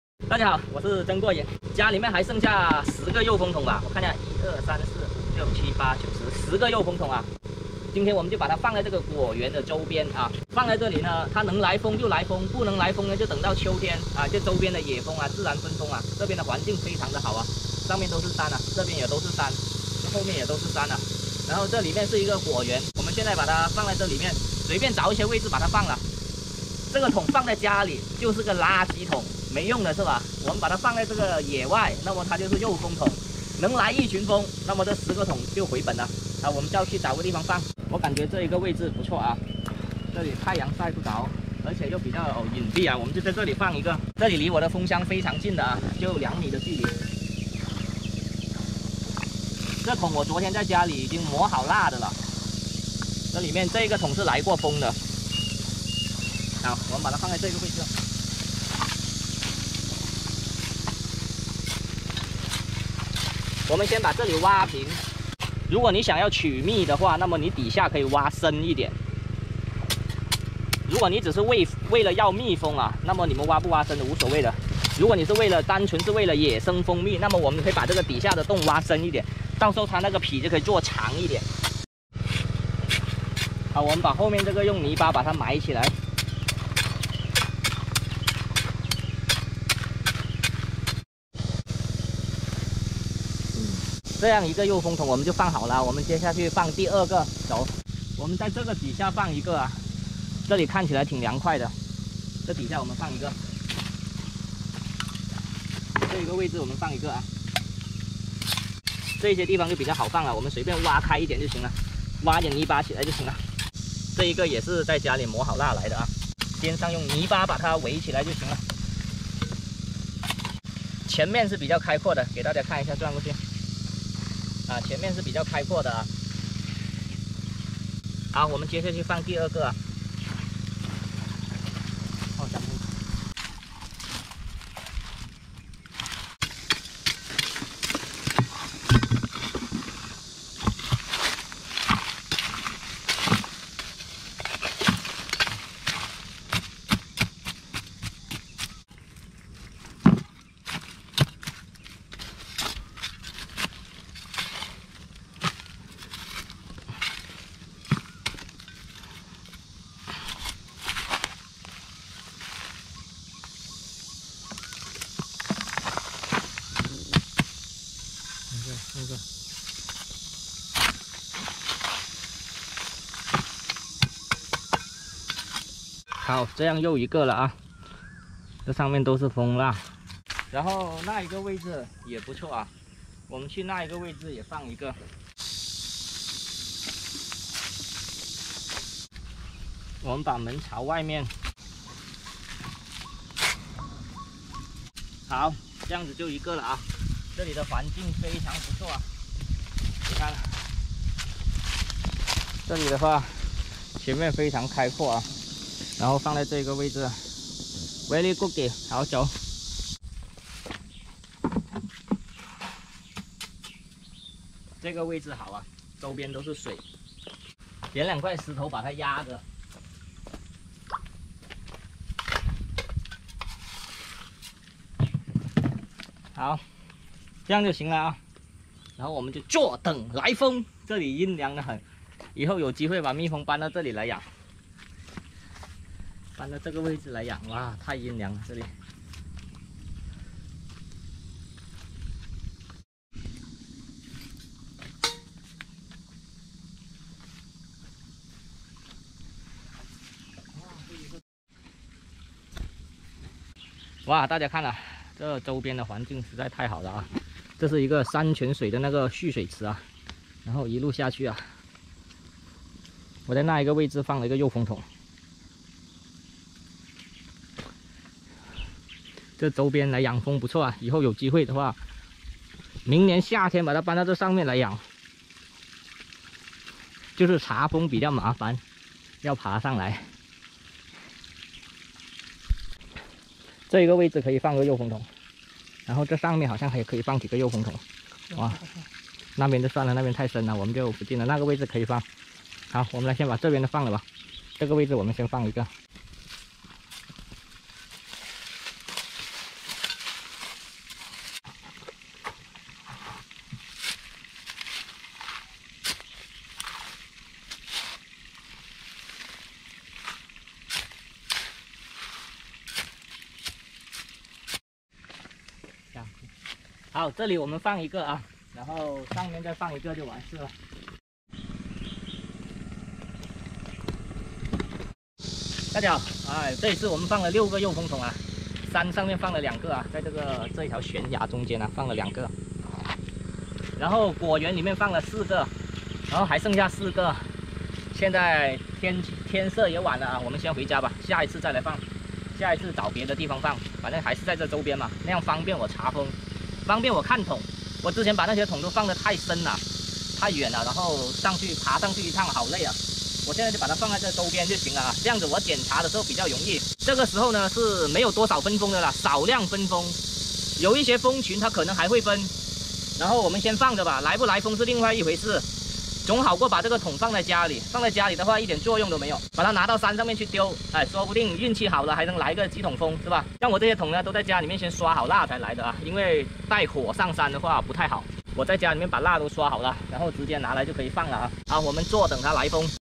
【真過癮】养蜂不买种，勤放诱蜂桶，小伙放点垃圾桶在果园周边等蜜蜂来居住
养蜂不买种，勤放诱蜂桶，小伙放点垃圾桶在果园周边等蜜蜂来居住

对于搬运我视频的人，我会维权举报。